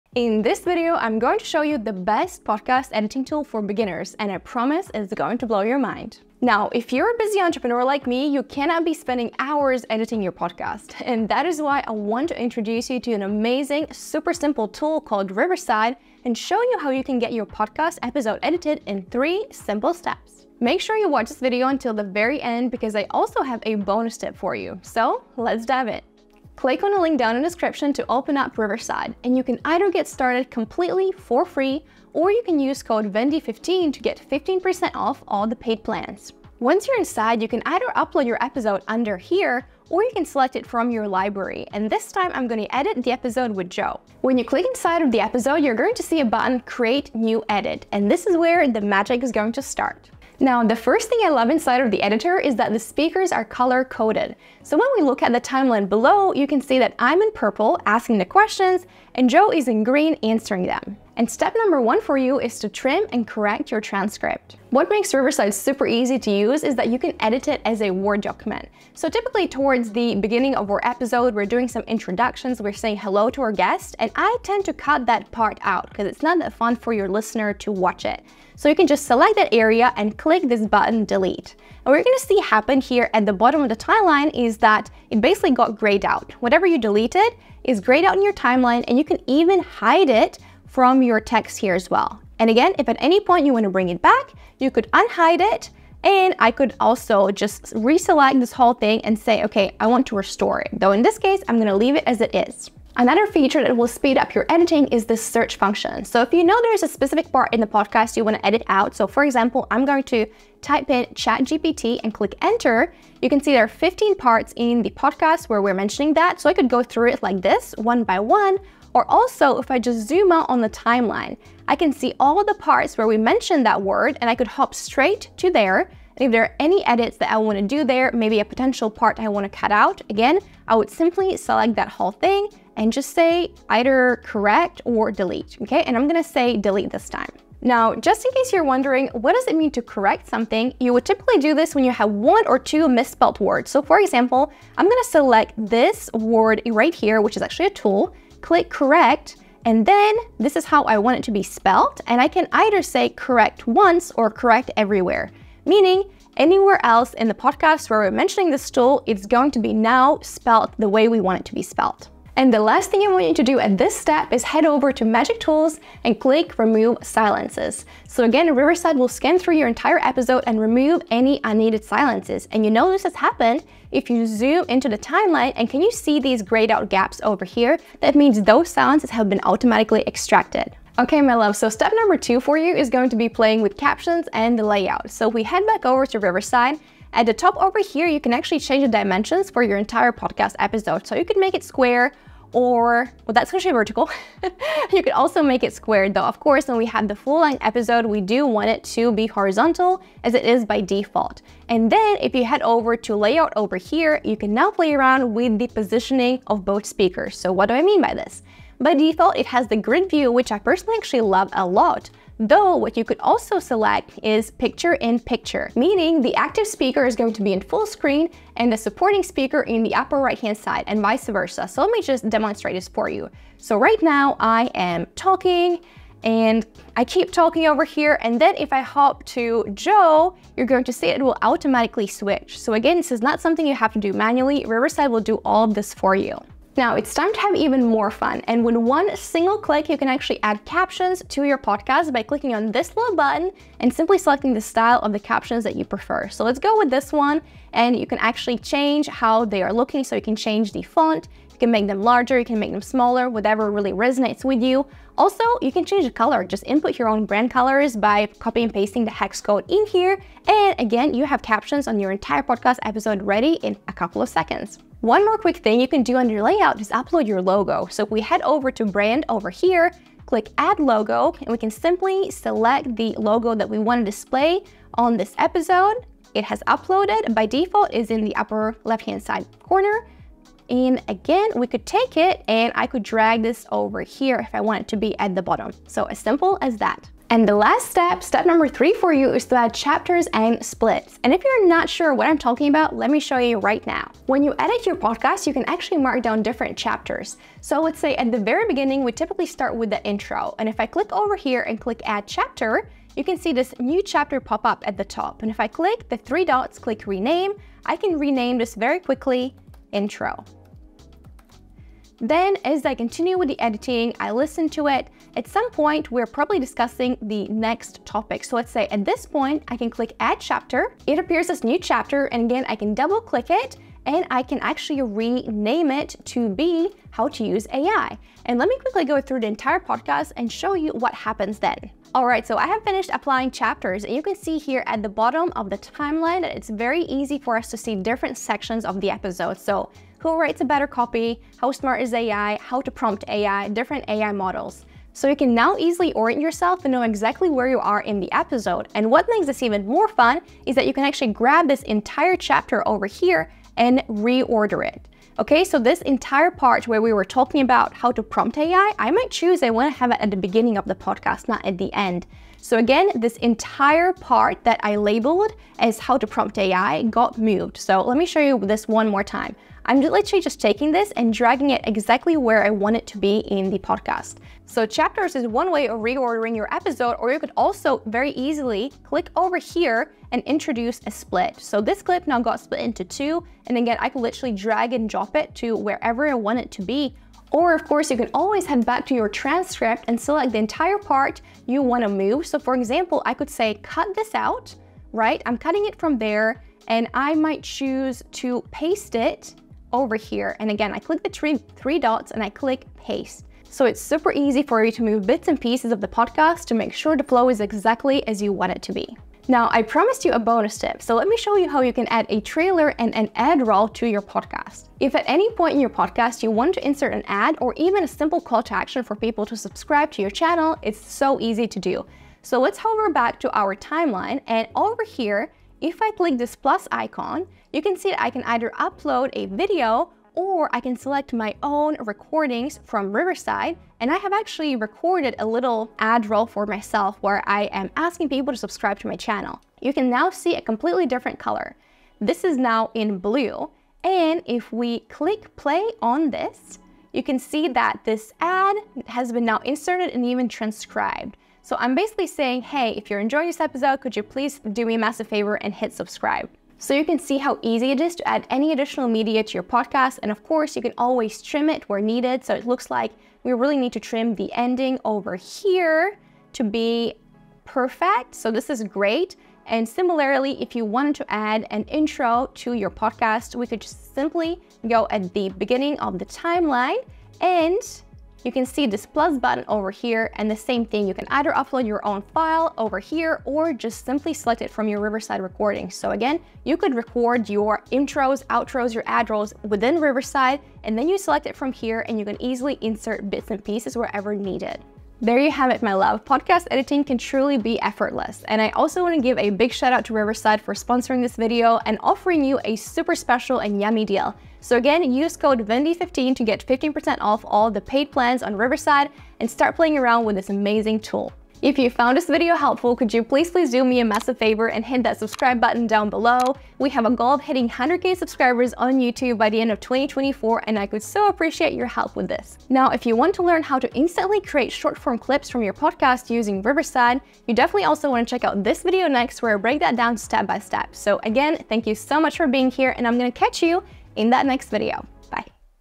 Easiest Podcast Editing Software 2024
*🤝 WORK WITH VENDY*

*🔗 WEBSITE & SOCIALS*

*🧰 TOOLS MENTIONED*

*📜 TIMESTAMPS:*

00:00 - Intro
01:02 - Step 1: Trim & Correct
03:02 - Step 2: Captions & Layout
04:48 - Step 3: Chapters & Splits
06:50 - Bonus: Trailer & Ad Roll
08:32 - Outro

*🌱 SUPPORT & ENGAGE*